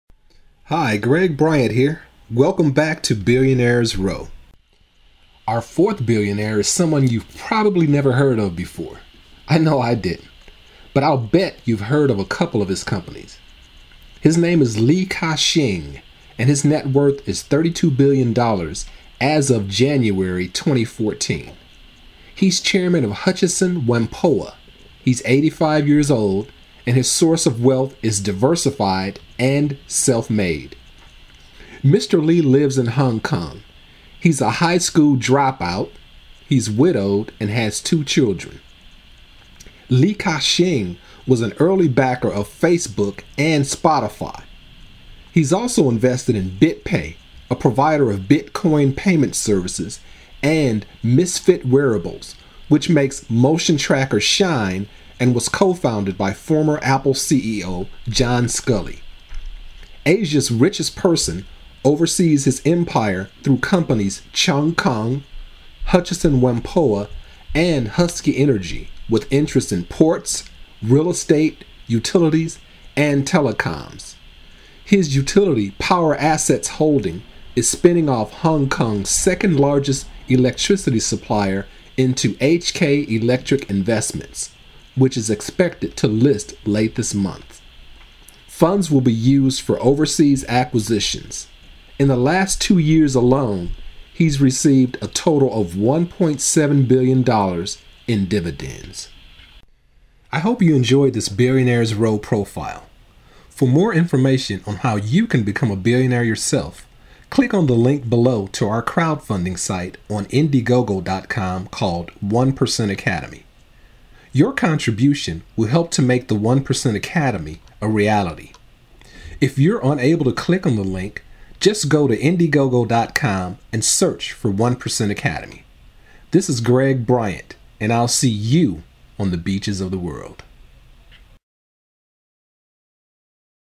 Billionaires Row - Li Ka-shing
One Percent Academy presents The Billionaires Row profile of Li Ka-shing.  If you can't beat the one percent why not join them?  Learn the 7 Simple Steps to wealth!